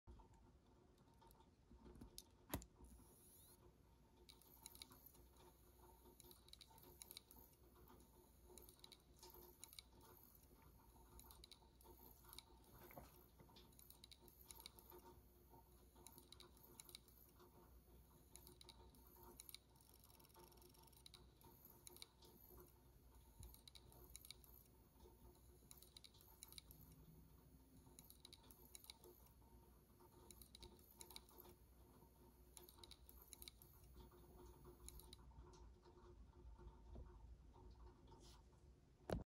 Inside of an iPod Hard Drive
This is the inner workings of an iPod classic 6th generation 120 GB.

In this video, the drive is searching for the OS to boot from, but due to a damaged plate, the iPod cannot boot.

This drive was damaged before I opened it up, so unfortunately I cannot show how the drive behaves during song skips or loading movies.